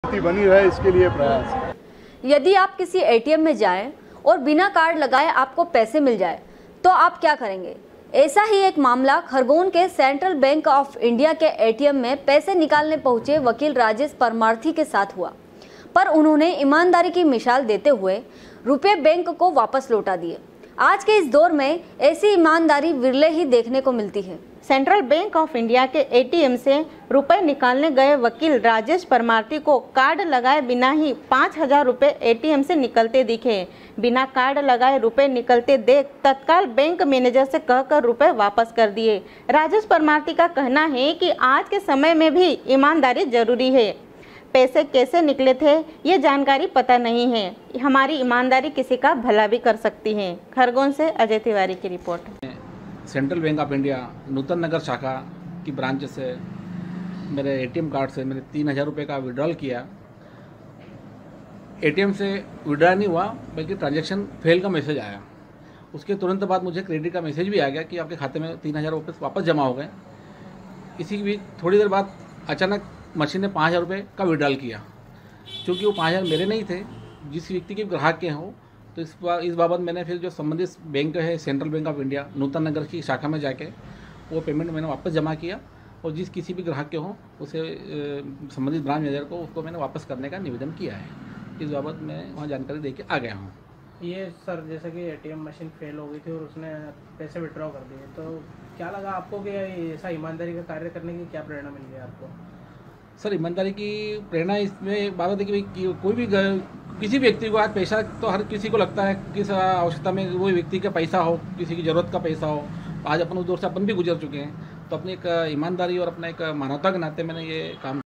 sw24news khargone, 15.07.2019
-यदि आप किसी एटीएम में जाए और बिना कार्ड लगाए ही आपको पैसे मिल जाए तो आप क्या करेंगे। ऐसा ही एक मामला खरगोन के सेंट्रल बैंक आॅफ इंडिया के एटीएम में पैसे निकालने पहुचे वकील राजेष परमार्थी के साथ हुआ। पर उन्होने ईमानदारी की मिषाल देतेे हुए रुपये बैंक को वापस लोटा दियें। आज के इस दौर में ऐसी ईमानदारी विरले ही देखने को मिलती हैsw24news khargone